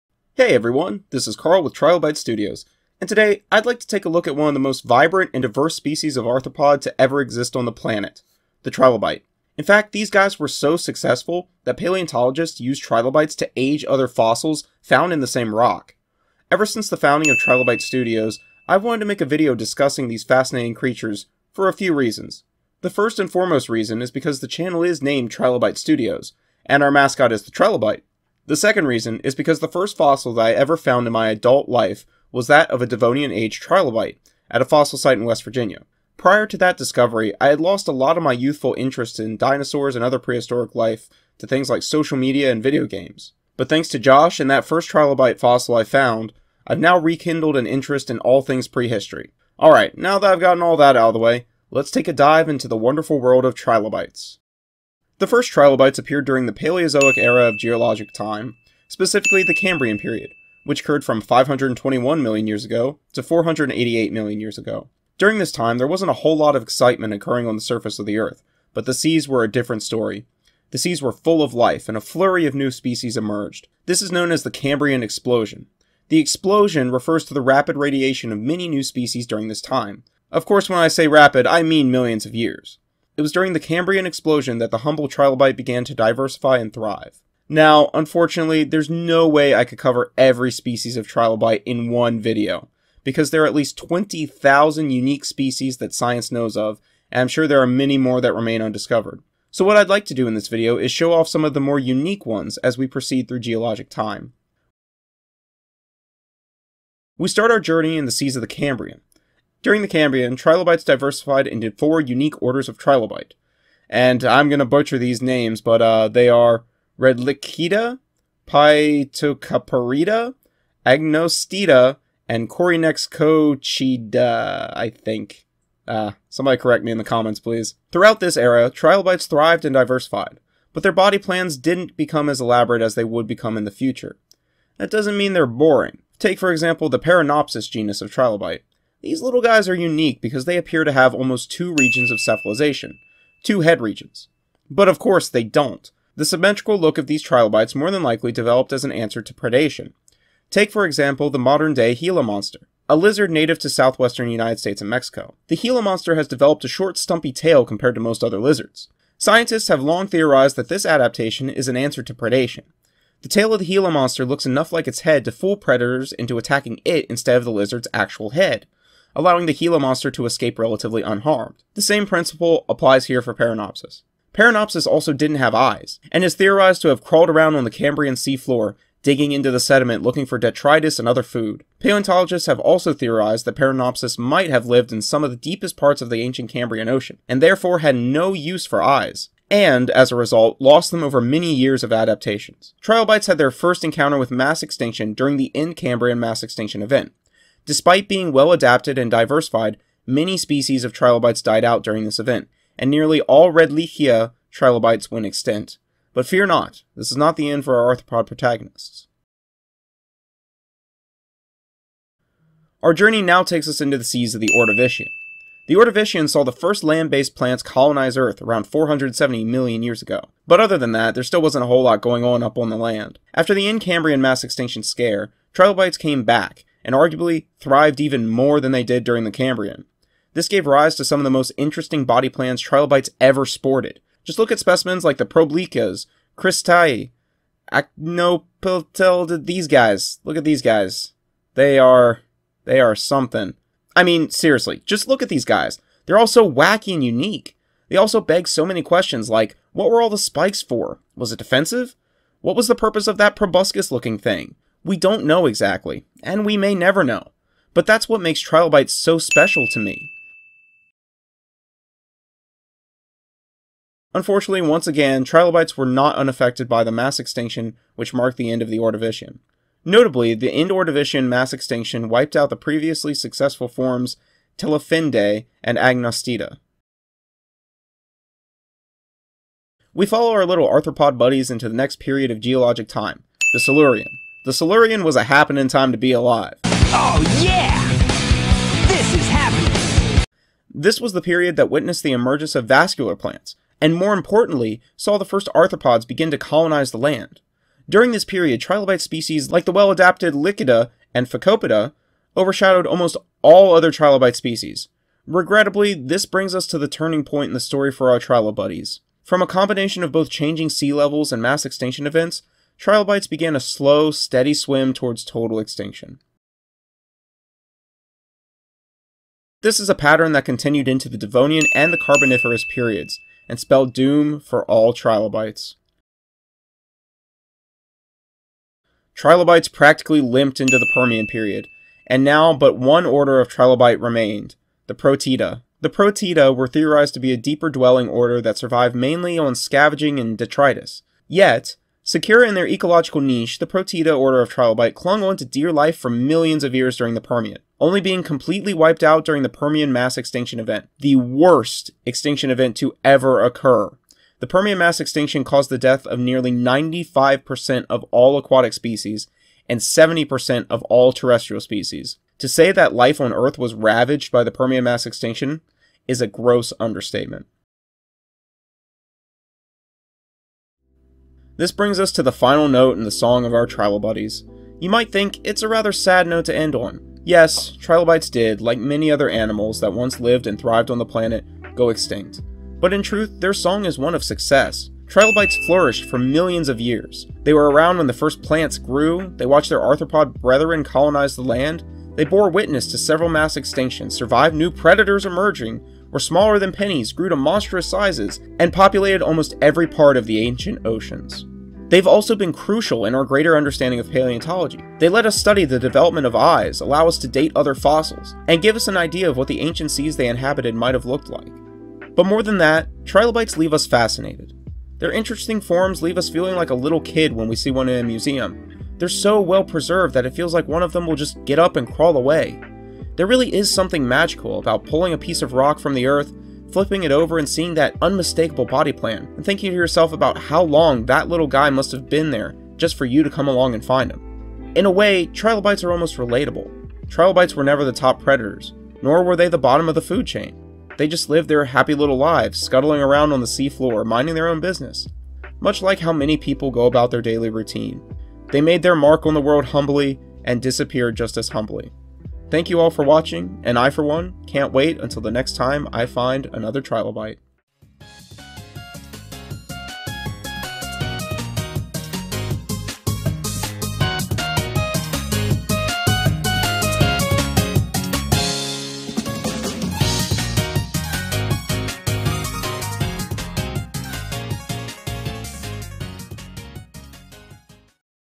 The Wonderful World of Trilobites
In today's video, Carl takes the viewer on a deep dive through the evolutionary wonder of one of Earth's most successful species, the Trilobite.

Please ignore these words, they are just here to help promote the videos.